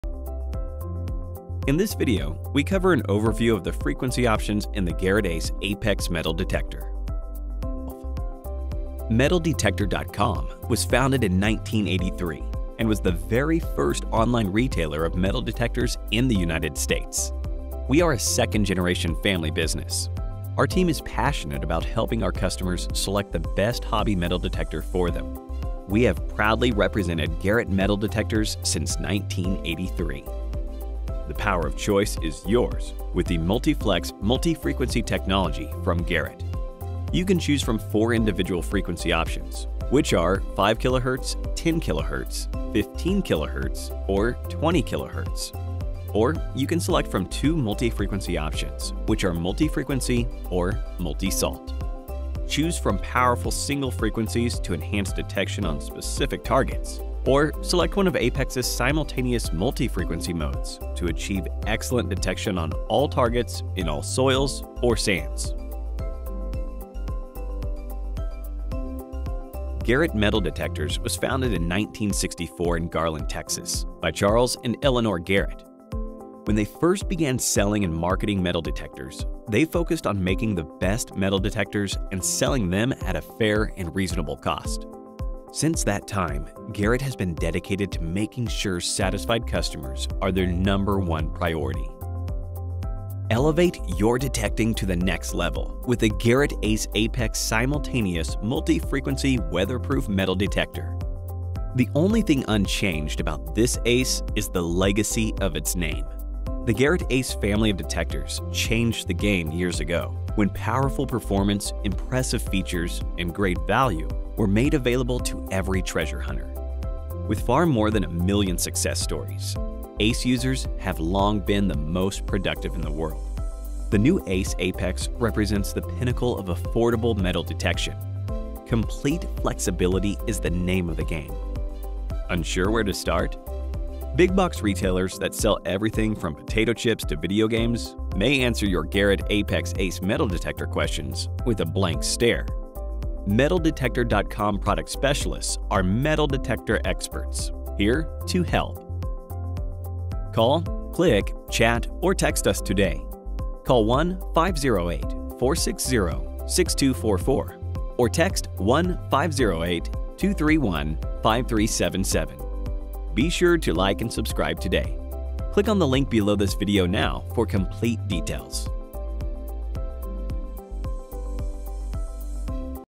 Garrett Ace Apex Metal Detector Frequency Options Overview
Garrett Ace Apex Wireless Garrett Ace Apex Basic In this video we cover an overview of the Frequency Options in the Garrett Ace Apex Metal Detector.